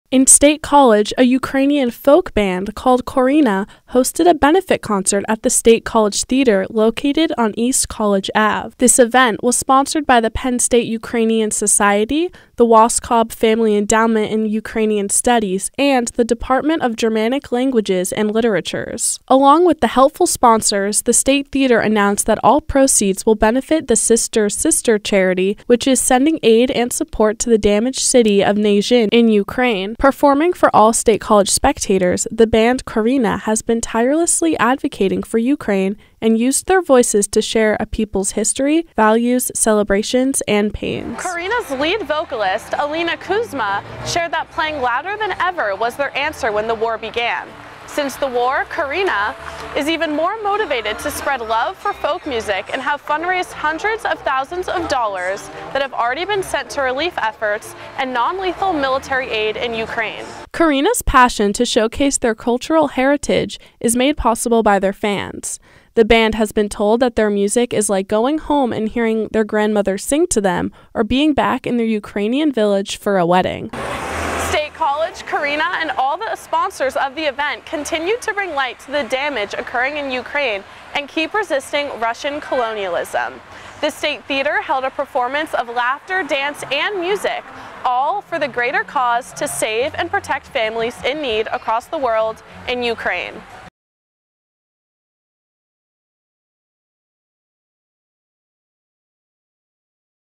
PSN News February 27, Benefit Concert for Ukraine
Field Reporter Natalie Simone gets an inside look at a benefit concert for Ukraine. Held downtown at the State Theatre, the folk band Korinya raised money for multiple different charities and organizations benefitting Ukraine. Take a look!